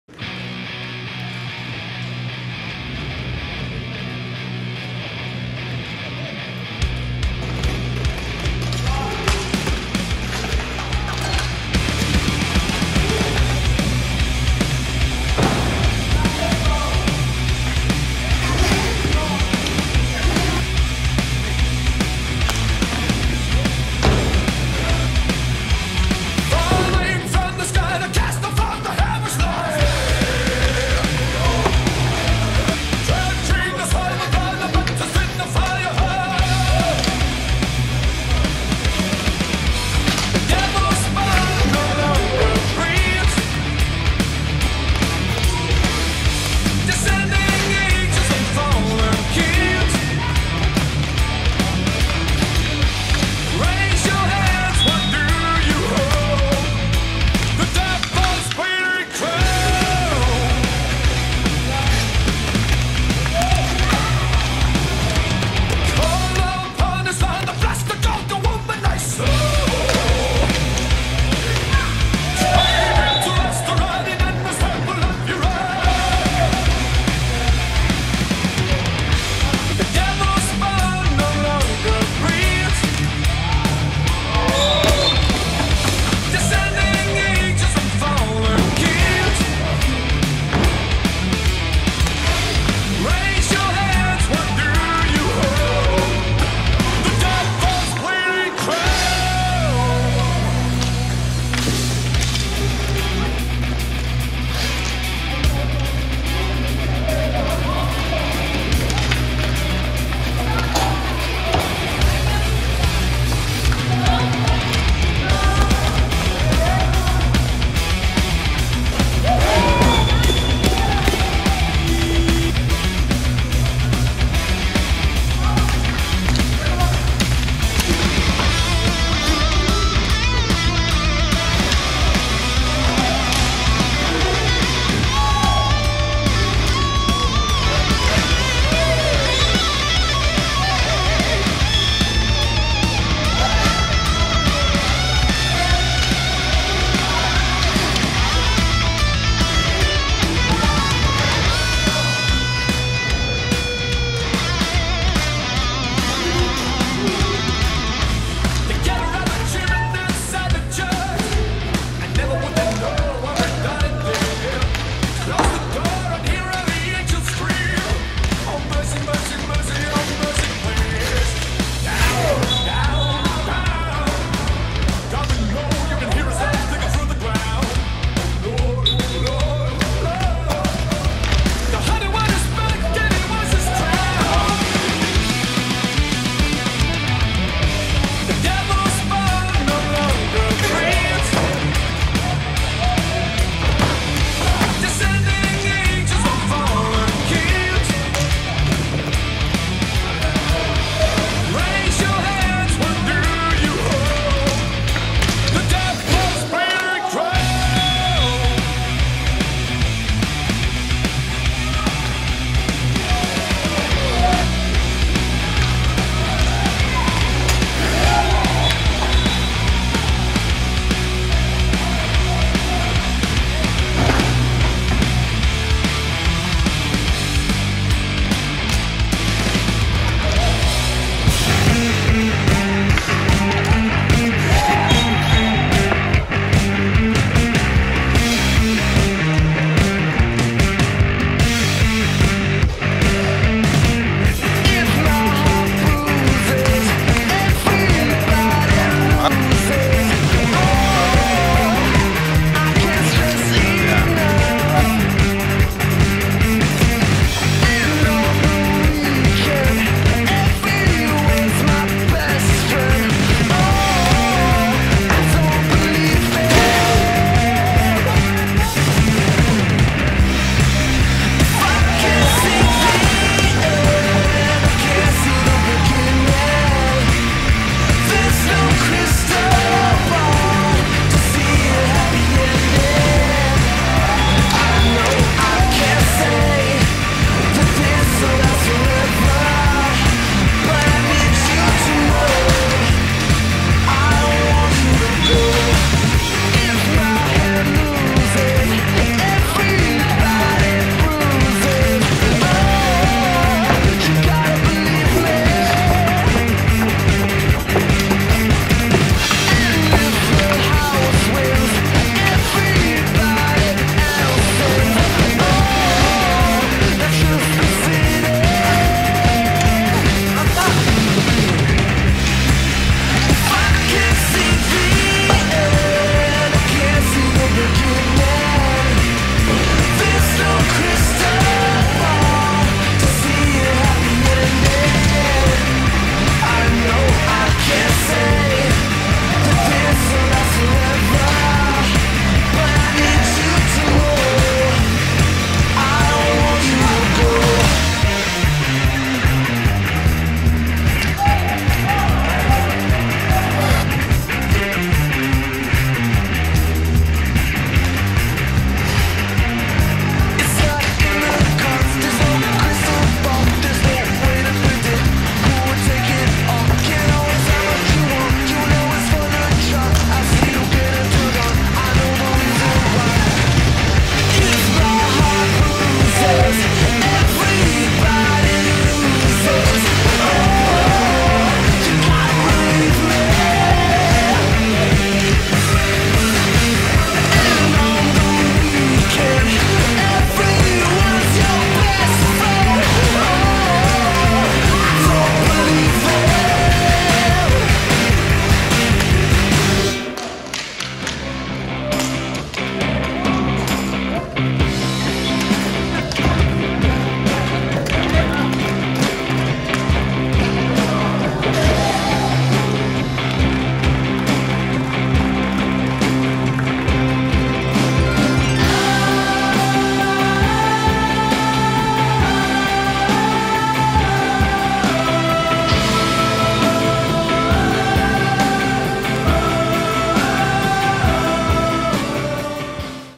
2019 LNC Uttigen
Première défaite de la saison pour notre LNC qui s'incline 4 à 6 contre Uttigen.